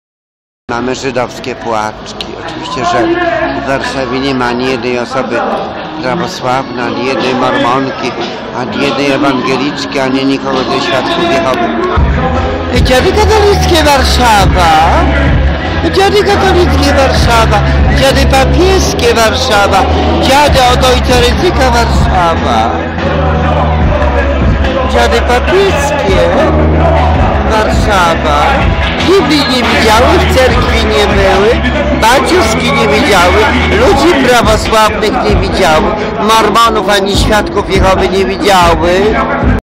DAMY RADĘ,GDY MAZUR ZA DORADZANIE NA STRONĘ PEPICZKÓW PRZEPĘDZIMY I PO LICHWIE!!!
CONCHITA RADIO EXTRA GDZIE NIE MA MIŁOŚCI,TO ŻADNA WIEDZA NIE MA WARTOŚCI//PUSTE I PRÓŻNE SŁOWA U LOCH NA ROZRODZIE 500+!!! PAŃSTWO DA//SPANIE ZA DARMO NAUKA ZA DARMO WSZYSTKO ZA DARMO,GDY LIDKA MUSIAŁA ODPRACOWAĆ KIEDYŚ,A DZIADUKI CHCĄ I CHCĄ//! DZIKA REPRYWATYZACJA MIESZKAŃ!/ZŁODZIEJE MIESZKAŃ//AFERA W WARSZAWIE!CWANIAKI MUSZĄ SPORO SIĘ UCZYĆ,ABY NABRAĆ KOGOŚ? TAK!!